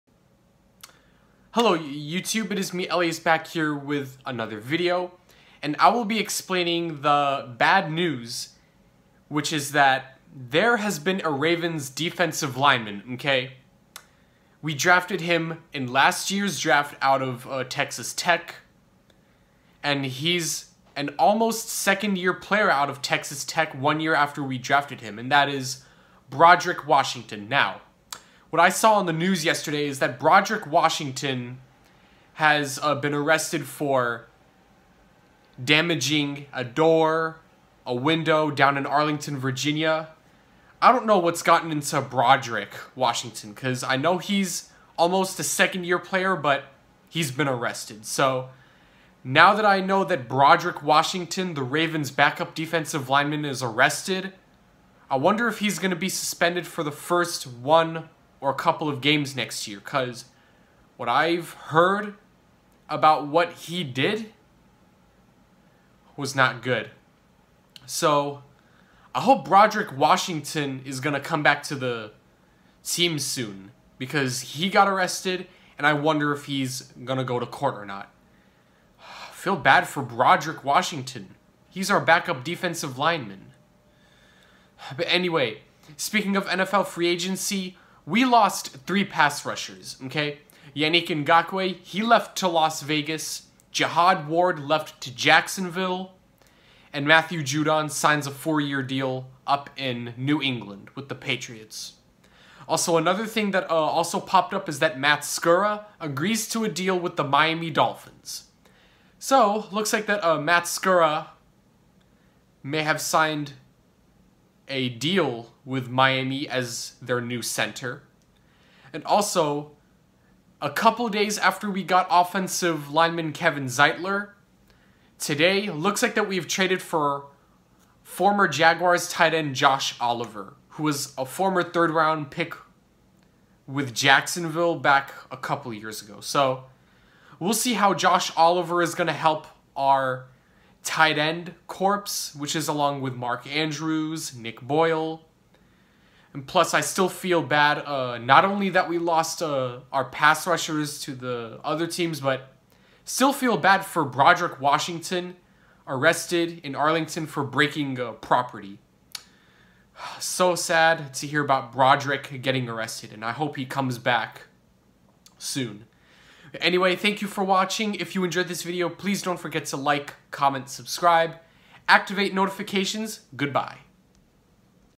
Broderick Washington aftermath/free agency update
If Broderick didn't get arrested, he would be alright, but still feel bad for him being arrested.